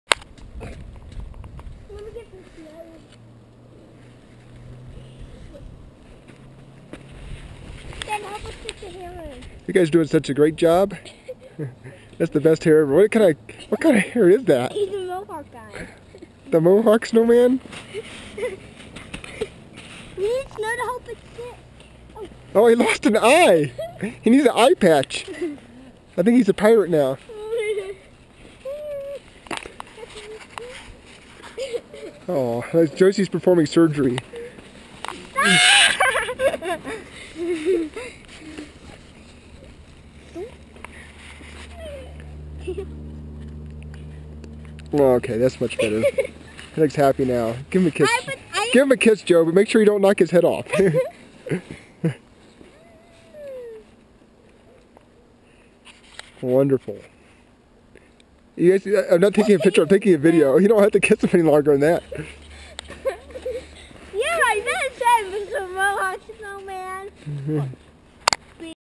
Sydney and Josie - First Snowman
Sydney and Josie put the finishing touches on their very first snowman.  Winter Park, CO